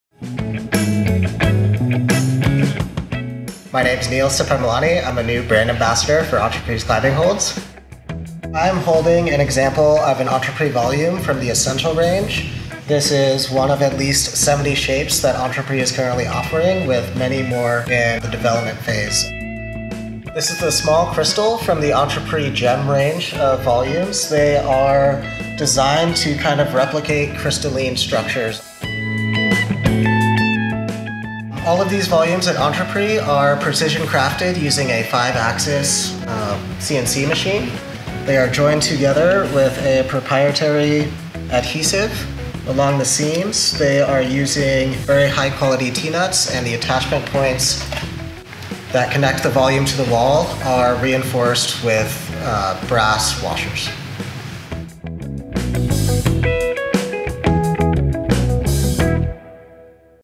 New EP Wood Volumes
Neal Sipahimalani talks you through a few new wood volume shapes.  We continue to make improvements to our wood volumes from friendlier texture to screw inserts to ergonomic round-overs.  We are continually drawing new shapes and putting them through our CNC machine for a little extra testing.  Keep an eye out for some curved nesting shapes launching soon!